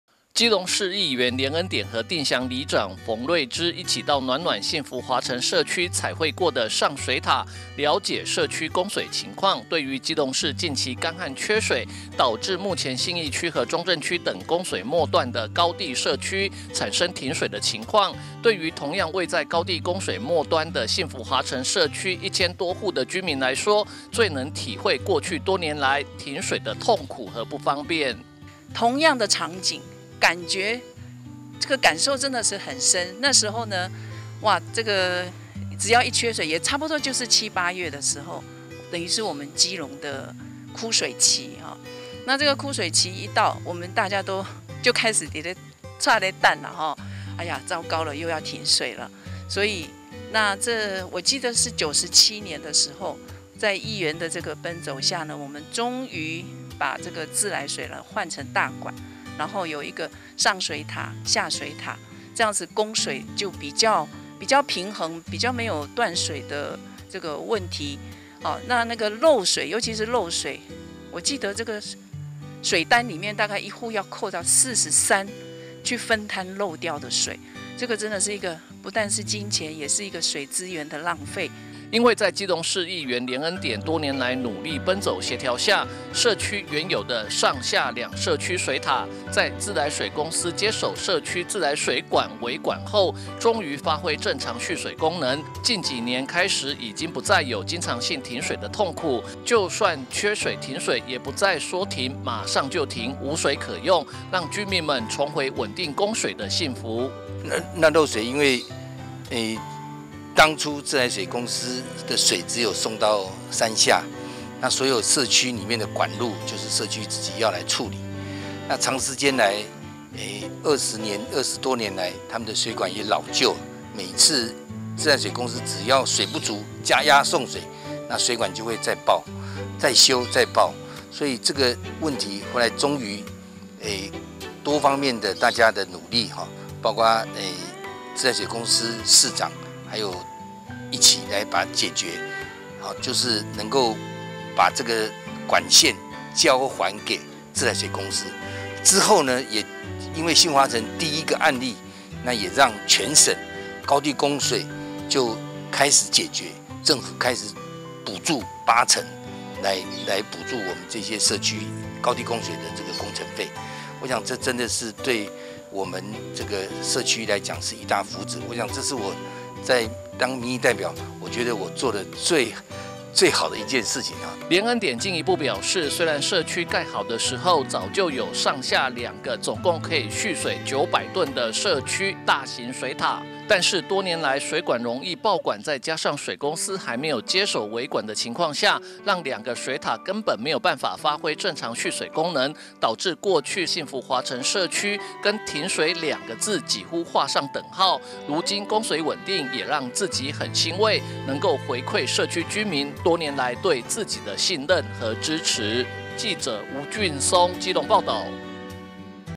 新聞 基隆|基隆缺水停水|基隆連日無雨新山水庫水位低|暖暖幸福華城社區|社區蓄水池配水池|基隆缺水 幸福華城不再遇停水就停 很幸福
基隆缺水 幸福華城不再遇停水就停 很幸福

(中嘉吉隆新聞記者吳俊松/基隆報導)基隆市目前面臨缺水的問題，導致信義區和中正區等供水末段的高地社區產生停水的情況，對於暖暖區幸福華城社區居民來說最能體會多年來停水的痛苦不便，幸福在基隆市議員連恩典多年奔走協調下，社區原有的上､下兩社區水塔發揮蓄水功能，近幾年開始已經不再有經常性停水的痛苦，讓居民們重回穩定供水的幸福！